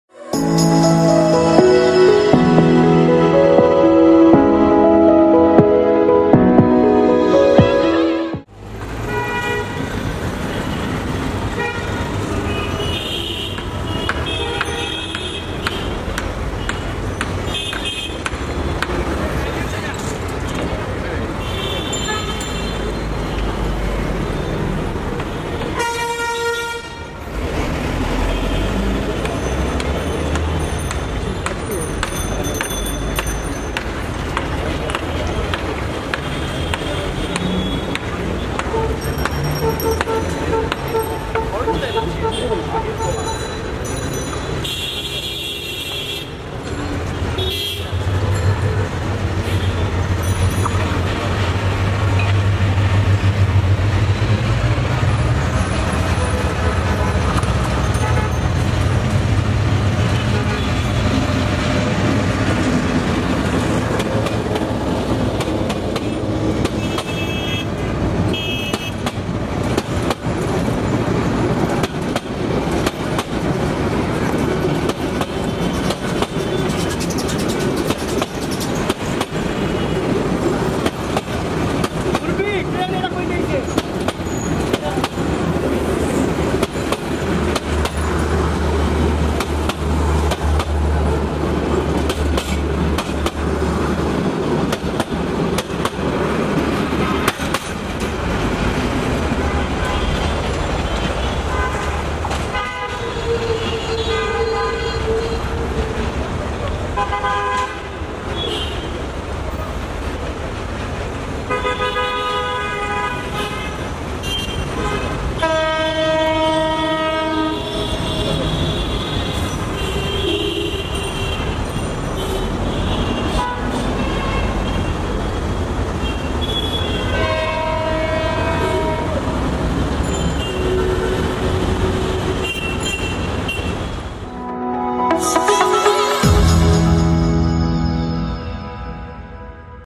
Jharia Jhangail To Dhaka Balaka Commuter train of Bangladesh Railway
Jharia Jhanjail To Dhaka Balaka Commuter train of Bangladesh Railway
Train Name: Balaka Commuter
Off Day- No
Departure From Dhaka 04:45
Arrival Jharia Jhangail 10:00